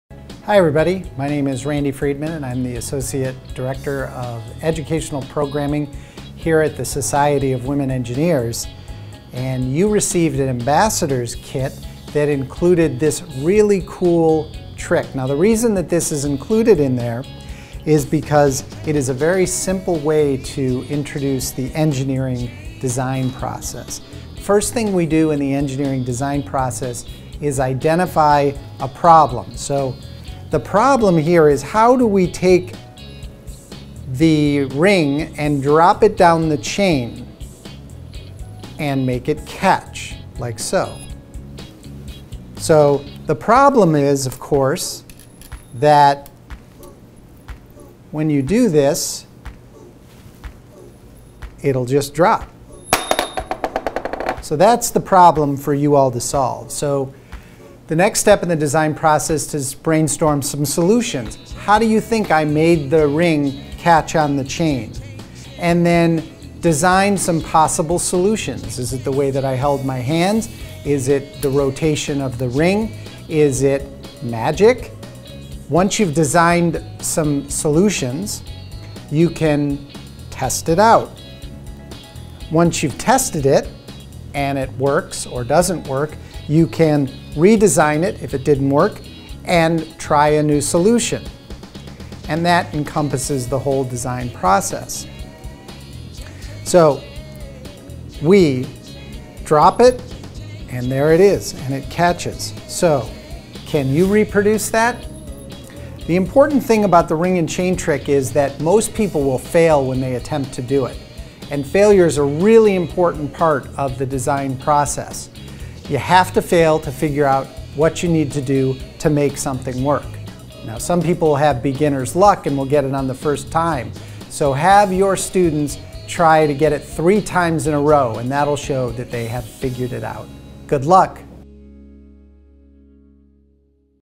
Ring and Chain Trick
Randy Freedman, Associate Director of Educational Programming at the Society of Women Engineers, explains how to use the ring and chain trick as a tool to teach students about the engineering design process.